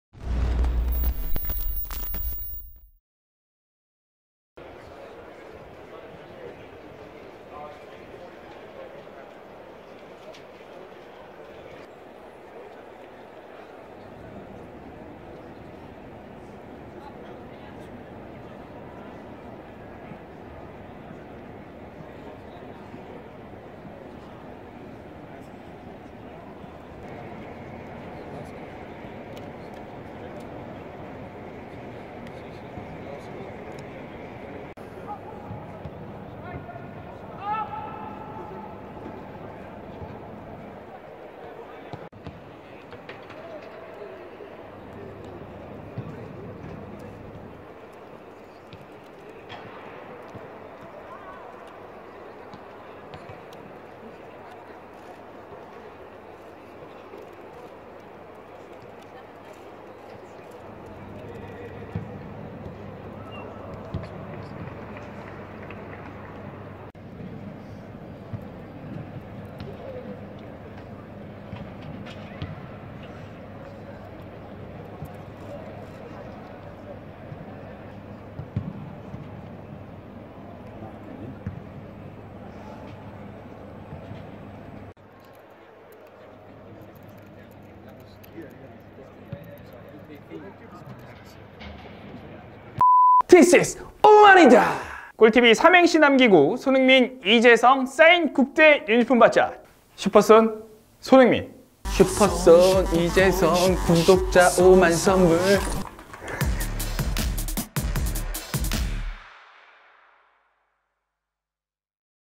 토트넘vs리버풀 챔스 결승 앞두고 한 마지막 트레이닝 훈련 엿보기! [GOAL LIVE]
챔스 결승 기다리기 지루하시죠~!?
그래서 준비했습니다. 토트넘과 리버풀의 마지막 트레이닝 현장
살짝 보여 드립니다!